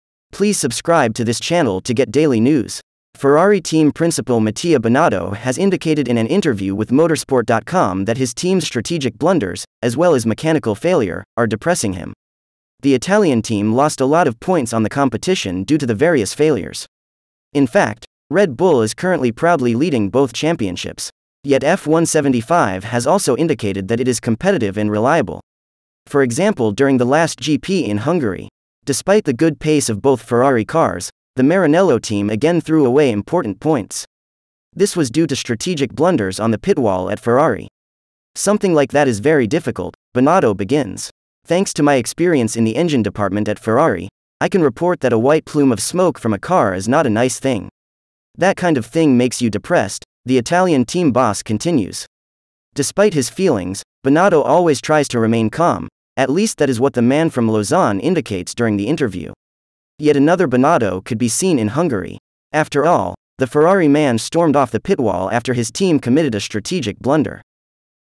Mattia Binotto in depression: 'Something like that makes you depressed' What happend with Ferrari f1
Ferrari team principal Mattia Binotto has indicated in an interview with Motorsport.com that his team's strategic blunders, as well as mechanical failure, are depressing him.

The Italian team lost a lot of points on the competition due to the various failures. In fact, Red Bull is currently proudly leading both championships. Yet F1-75 has also indicated that it is competitive and reliable. For example during the last GP in Hungary. Despite the good pace of both Ferrari cars, the Maranello team again threw away important points. This was due to strategic blunders on the pit wall at Ferrari.

“Something like that is very difficult,” Binotto begins. “Thanks to my experience in the engine department at Ferrari, I can report that a white plume of smoke from a car is not a nice thing. That kind of thing makes you depressed," the Italian team boss continues. Despite his feelings, Binotto always tries to remain calm, at least that is what the man from Lausanne indicates during the interview. Yet another Binotto could be seen in Hungary. After all, the Ferrari man stormed off the pit wall after his team committed a strategic blunder.